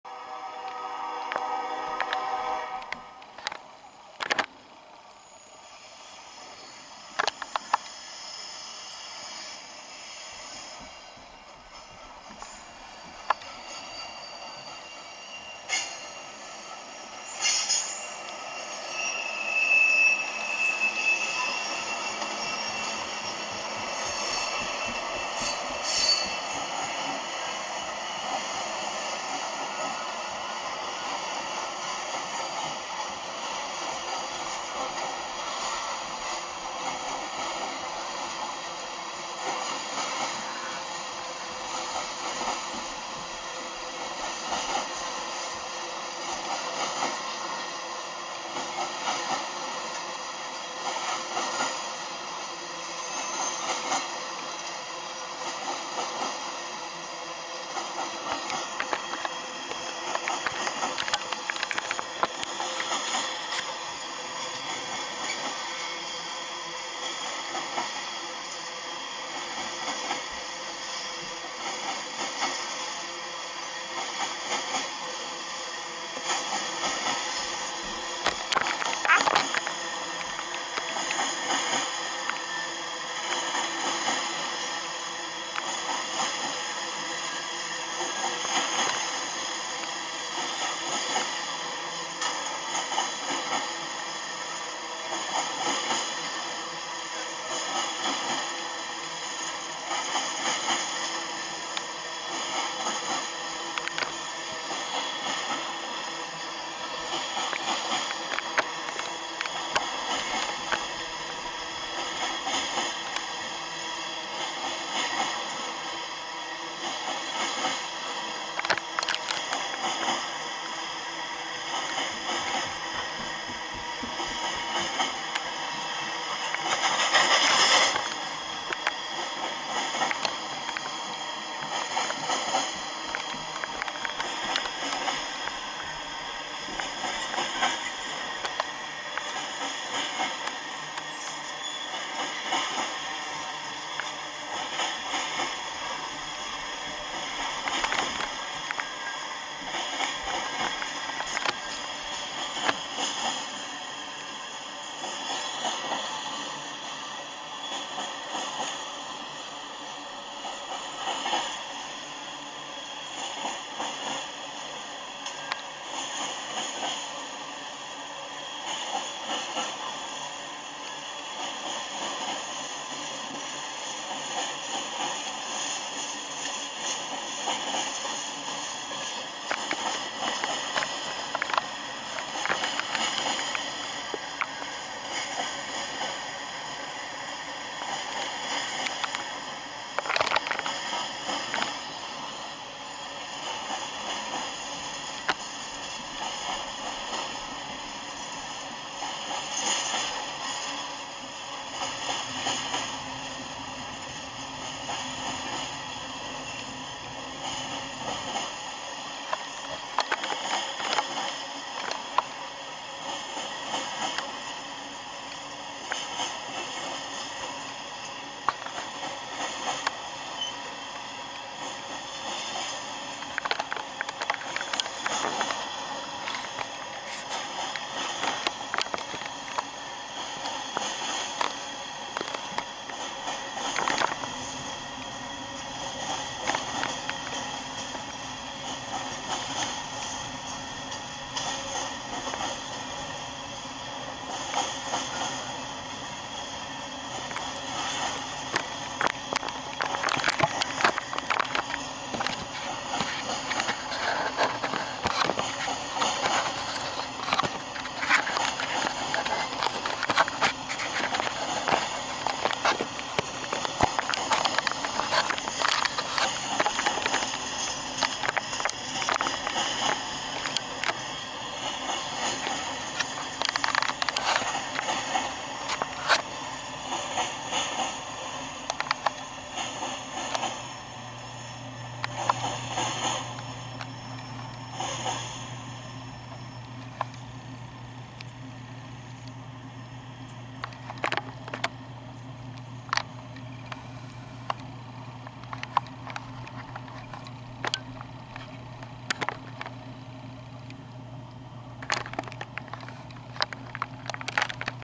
UP Trio Leads Slow NS 65Q on Elkhart IN PTZ Cam
As the afternoon flies by & a few hours after NS 310 Had a another Derailment on the NS Chicago Line in Amherst OH... Slow Westbound NS Empty Tank Train 65Q Passes by The Elkhart IN PTZ Cam with a UP Trio in the Lead

NS 65Q (Jacksonville FL - Chicago IL)